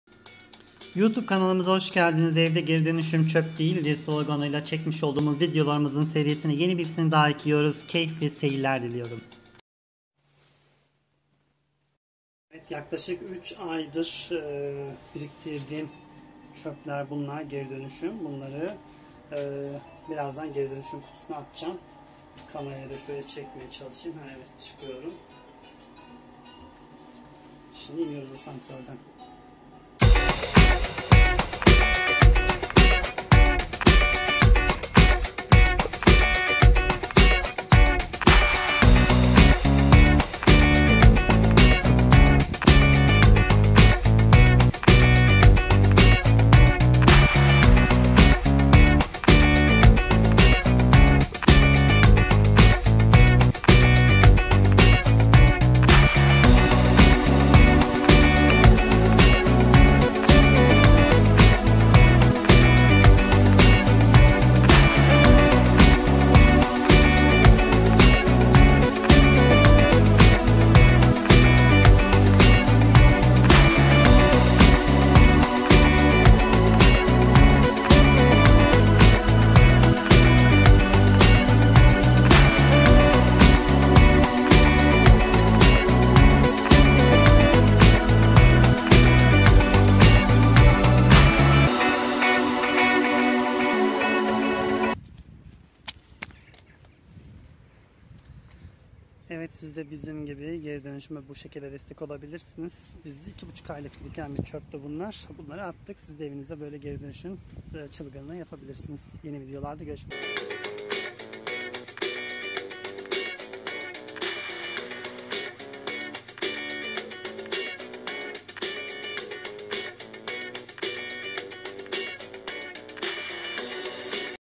Evde Geri Dönüşüm!!! 3 Torba Çöp Çıktı !!! (5.Video)
Bir kaç yıldır evde biriktirdiğimiz geri dönüşümleri çöpe değil, tekrardan ekonomiye kazandırma gayretlerimiz sürüyor, uzun bir aradan sonra tekrardan bir video çekerek sizleri bilinçlendirme gayretimiz devam ediyor.Sizlerde evinizde bizim gibi geri dönüşüm kutusu köşesi yaparak, çöplerinizi geri dönüşüme kazandırabilirsiniz.Sizlere ilham vermesi için sitemizde bunu uzun uzun anlattım, bizlerde sizin gibiydik günün birinde kafamızda bir ışık çaktı ve geri dönüşüm sever oluverdik, nasıl mı olduk şuraya tıklayın